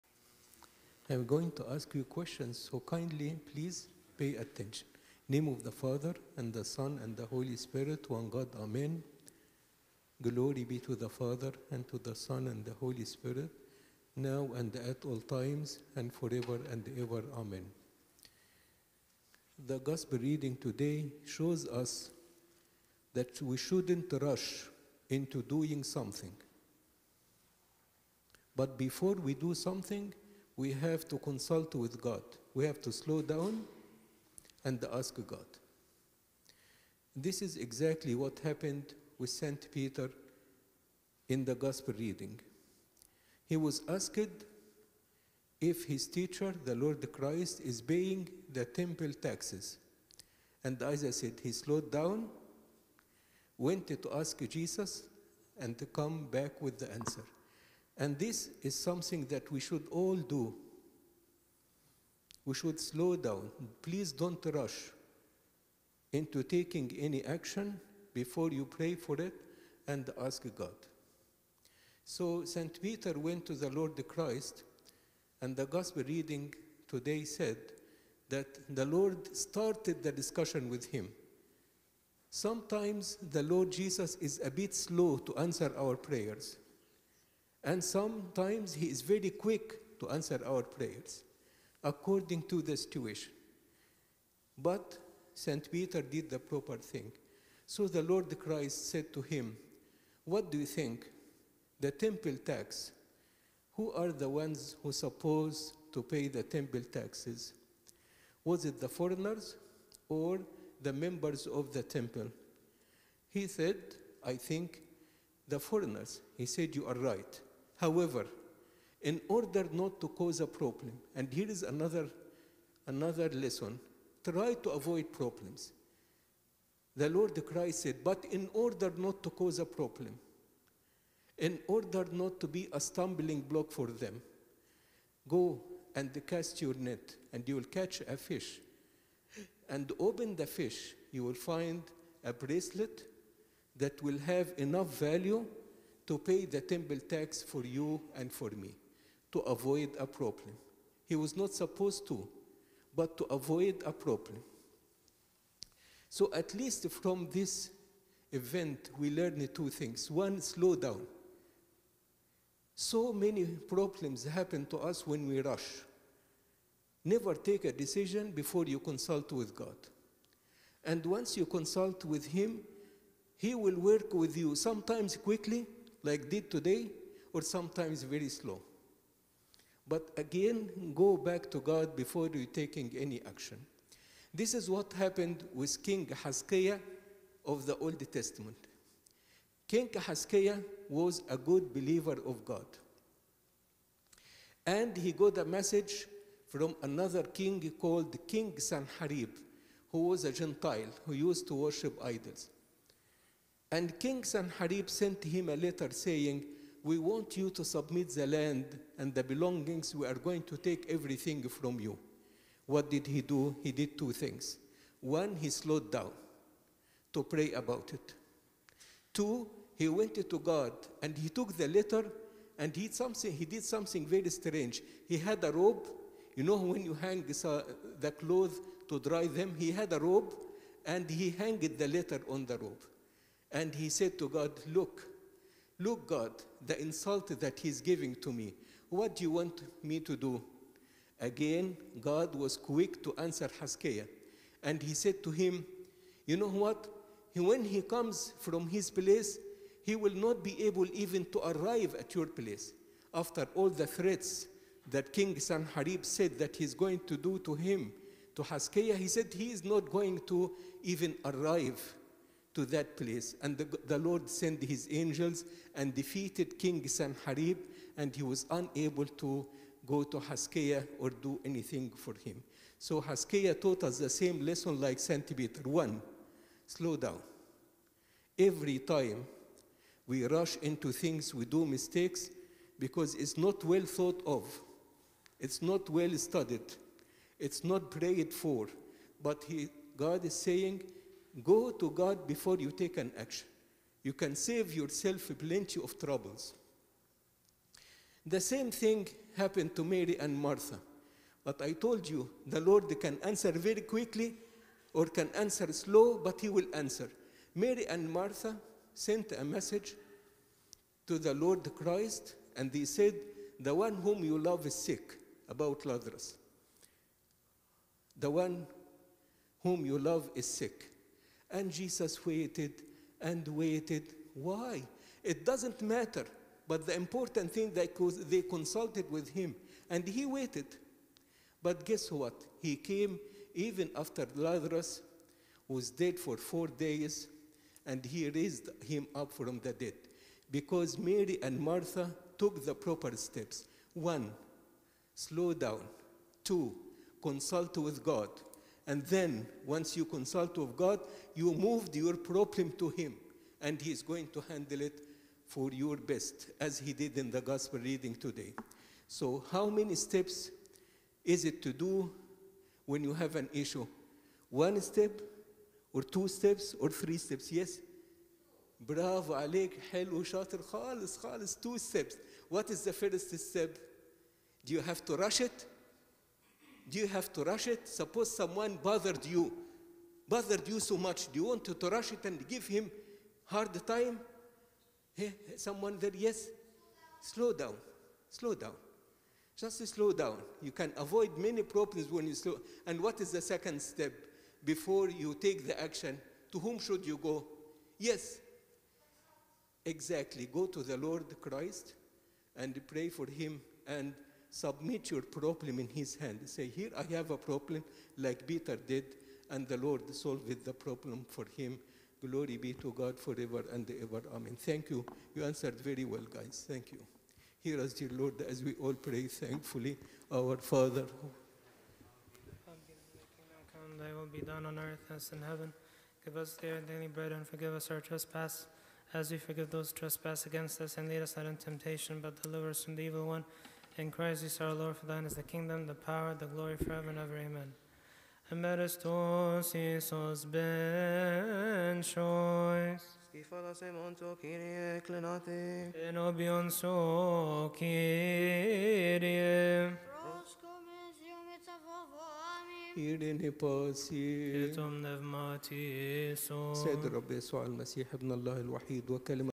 17 October 2020 - Fr.Rueiss Saturday Vespers Prayers Sermon: "Take God's opinion before you act"
St. George & St. Rueiss Coptic Orthodox Church